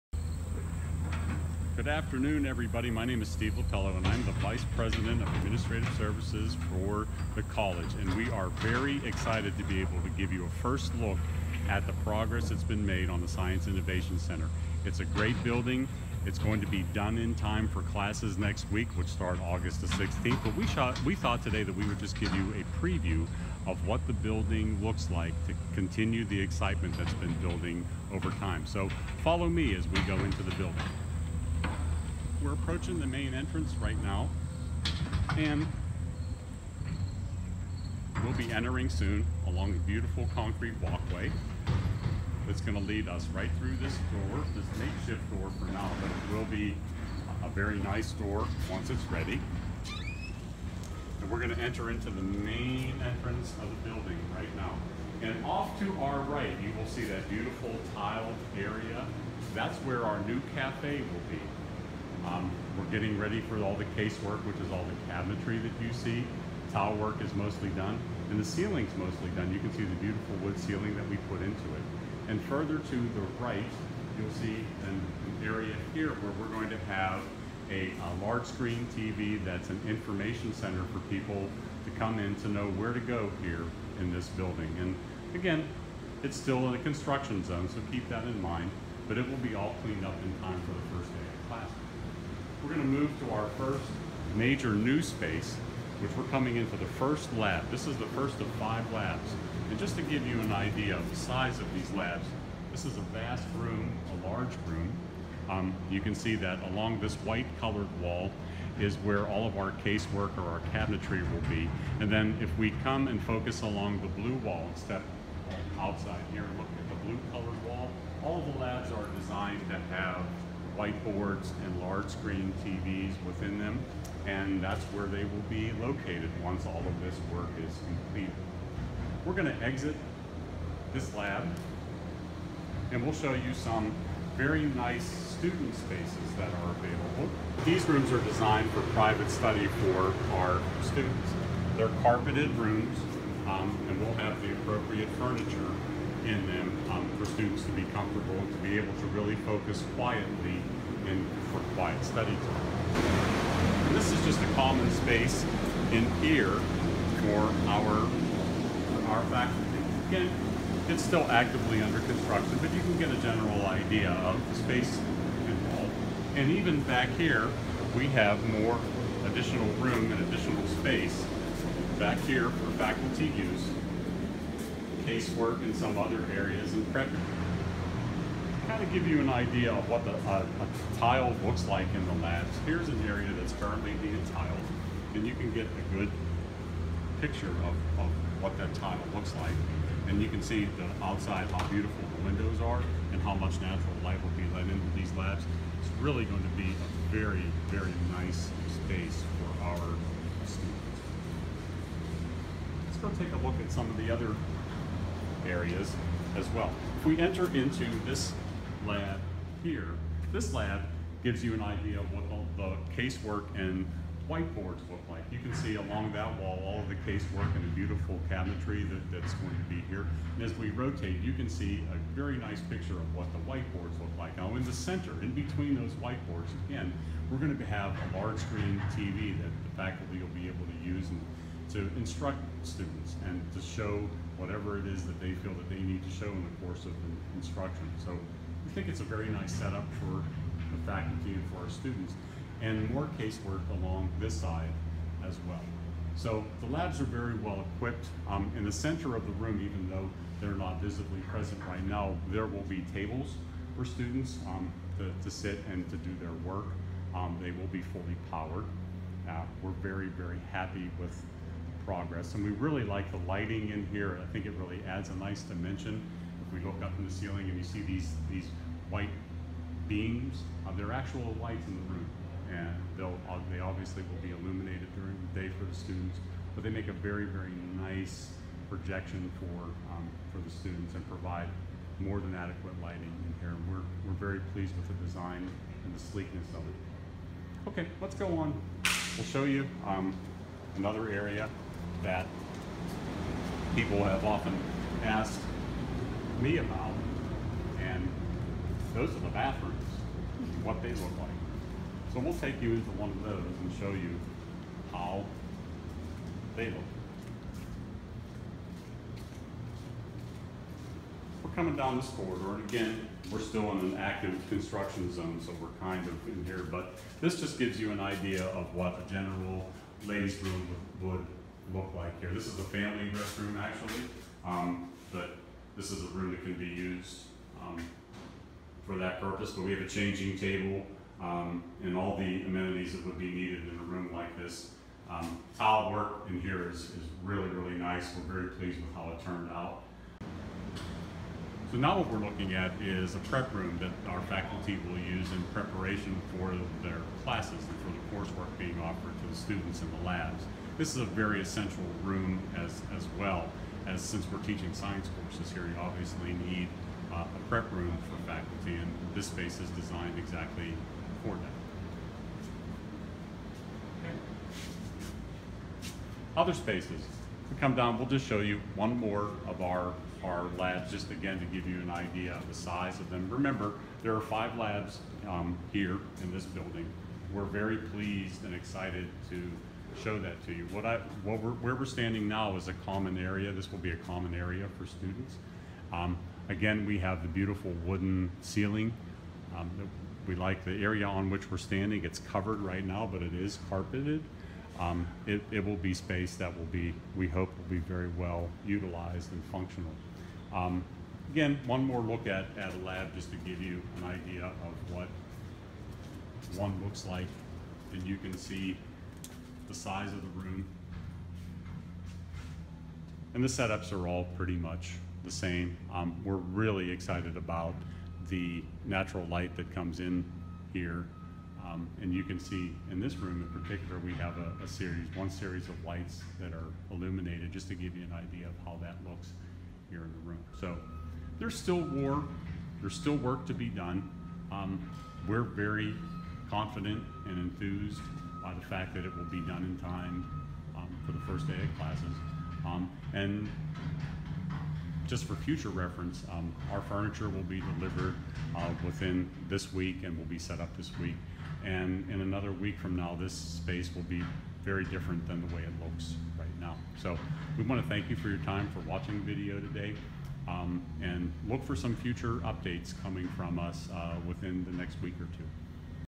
Science Innovation Center tour (8/9/18)
We go inside for a preview one week before it opens for classes.